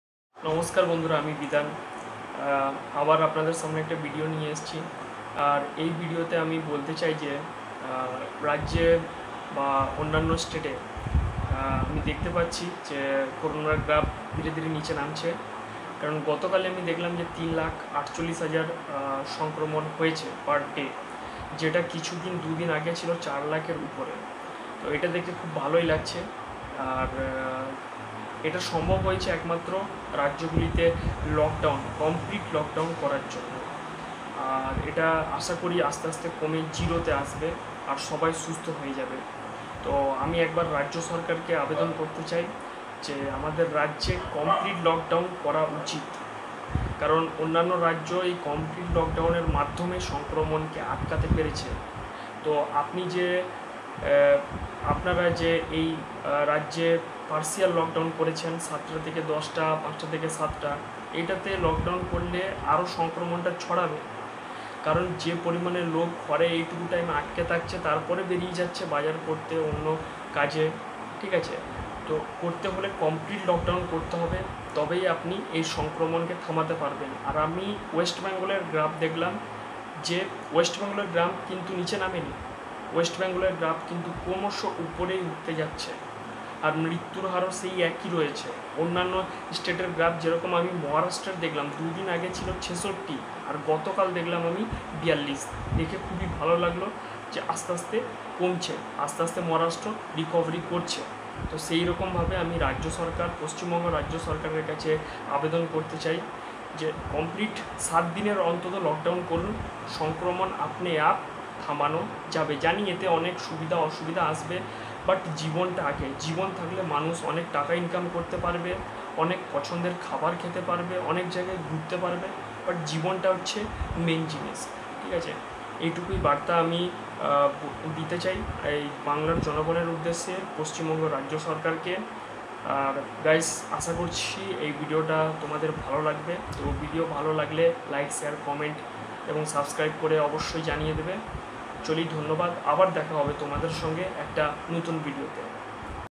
what should be the complete lockdown in west bengal ll Bidhan Chandra Das
✅ welcome to my chhanal
     Bidhan Chandra Das

      Bidhan Chandra Das

আমি পশ্চিমবঙ্গ রাজ্য সরকারের উদ্দেশ্যে কিছু বার্তা বলেছি এই ভিডিওর মাধ্যমে,
কারণ আপনি দেখতে পাবেন যেইসব রাজ্য কমপ্লিট লকডাউন আছে সেই সব রাজ্যের করুনার গ্রাফ ক্রমশ নিচে নামছে,

সেই জন্যই আমি পশ্চিমবঙ্গ রাজ্য সরকারের কাছে আবেদন জানাতে চাই যে রাজ্যে সাত দিন অন্তত কমপ্লিট লকডাউন করা হোক,

জানি এতে অনেক সুবিধা অসুবিধা হবে অনেক লোকের প্রবলেম ফেস করতে হবে কিন্তু মানুষের জীবনটা আগে, যদি জীবন থাকে তাহলে অনেক টাকা জীবনে রোজগার করতে পারবে অনেক ভাল-মন্দ খাবার খেতে পারবে অনেক ভালো ভালো জায়গায় ঘুরতে যেতে পারবে,

আবার দেখা হবে একটা নতুন ভিডিওতে ধন্যবাদ ভিডিওটা দেখার জন্য,, ভাল থাকবেন সুস্থ থাকবেন।